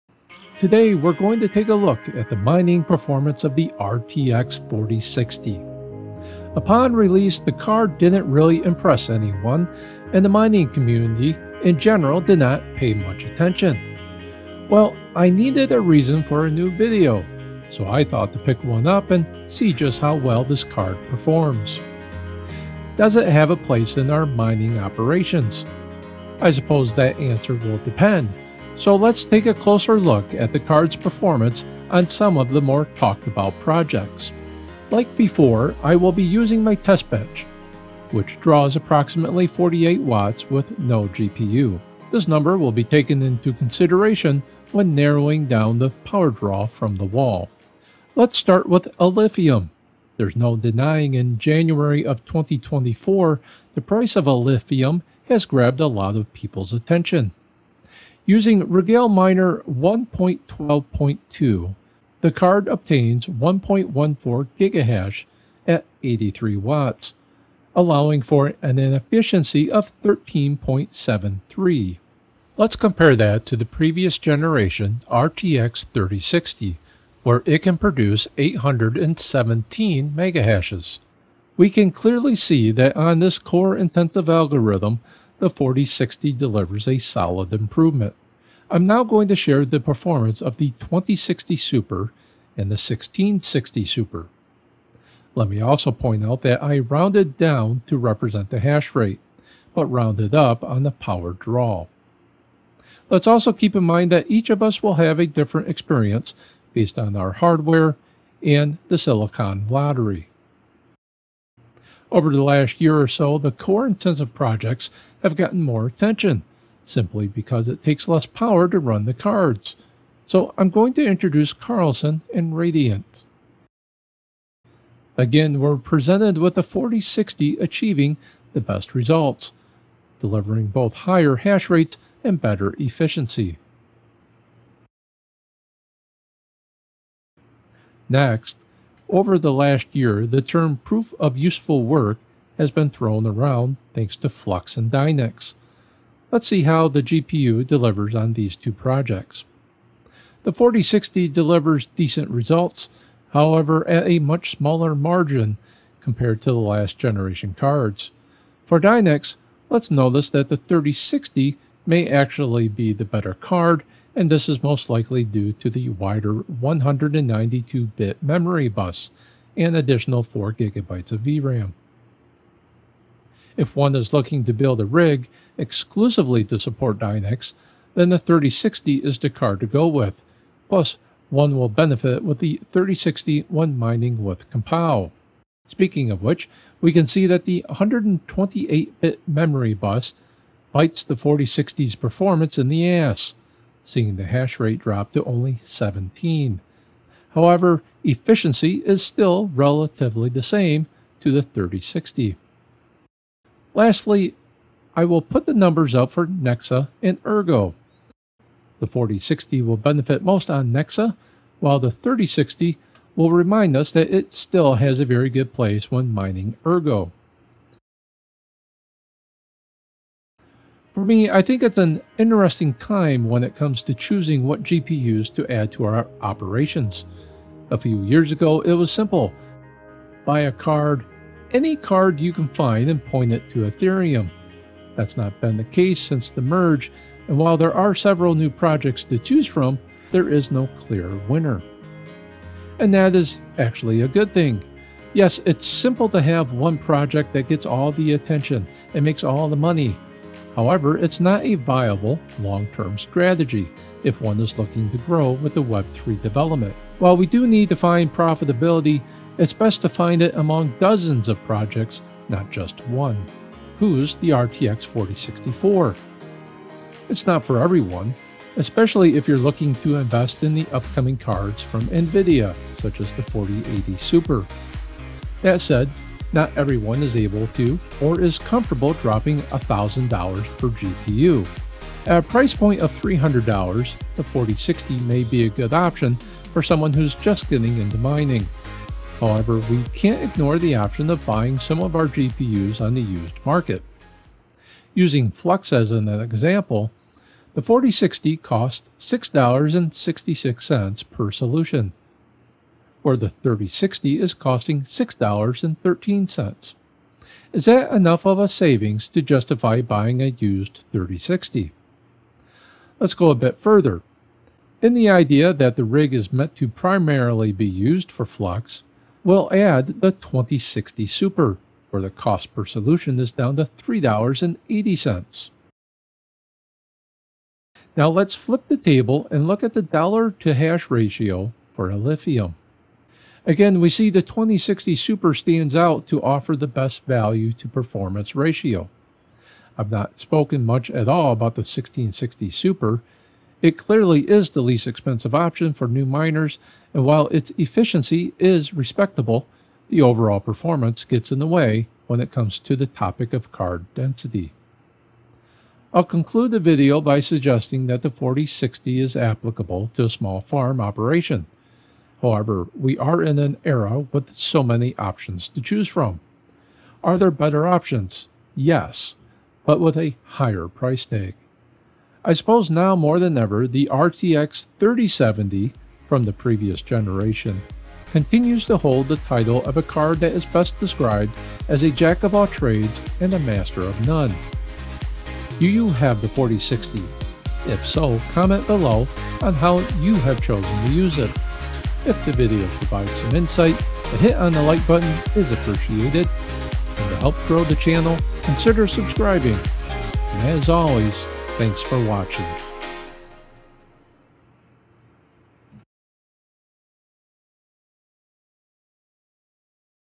Mining With The RTX 4060
Should the RTX 4060 be added to your mining operations?  Let's take a review of the card on some of the more popular projects such as Flux and Alephium.